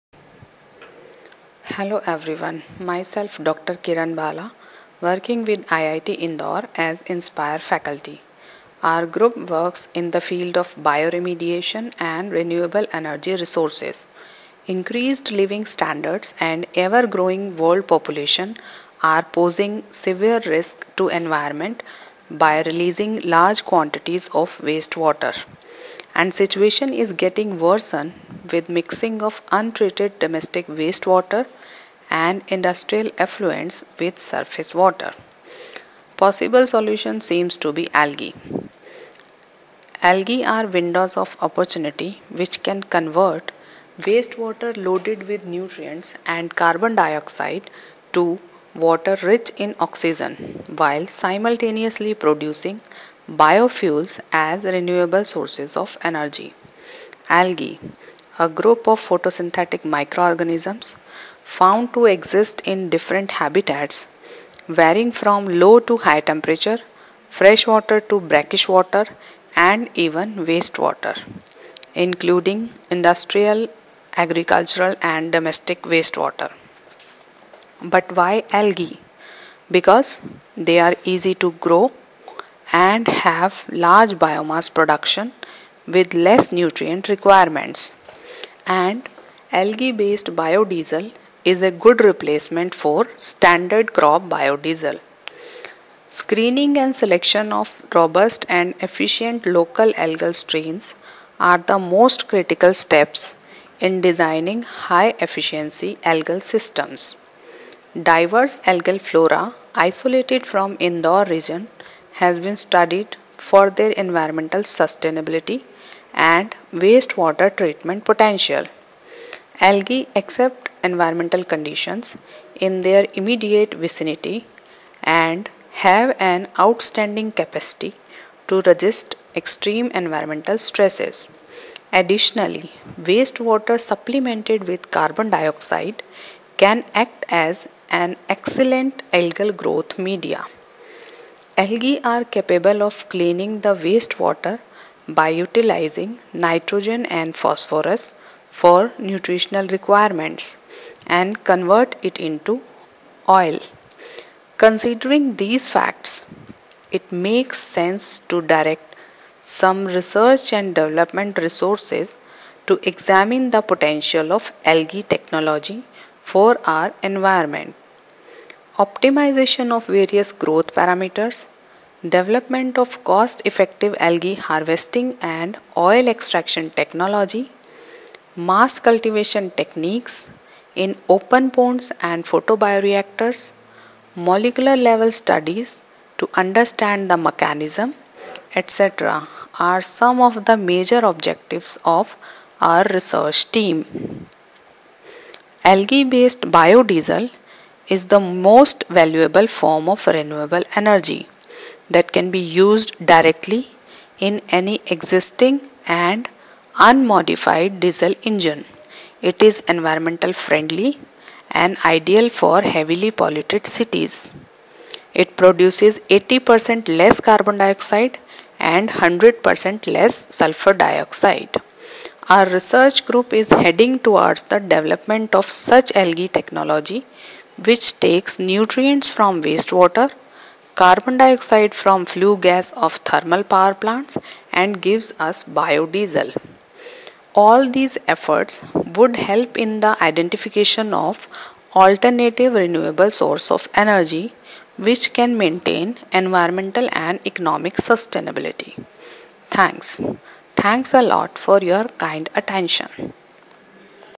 Kiran Bala, IIT Indore, India
Title of video entry: Algae based solution to water pollution

The idea: Wastewater treatment using algae

Full name: Kiran Bala

Age: 34

Highest Academic Degree: Ph. D.

Research field and research focus: Phycoremediation and biofuels development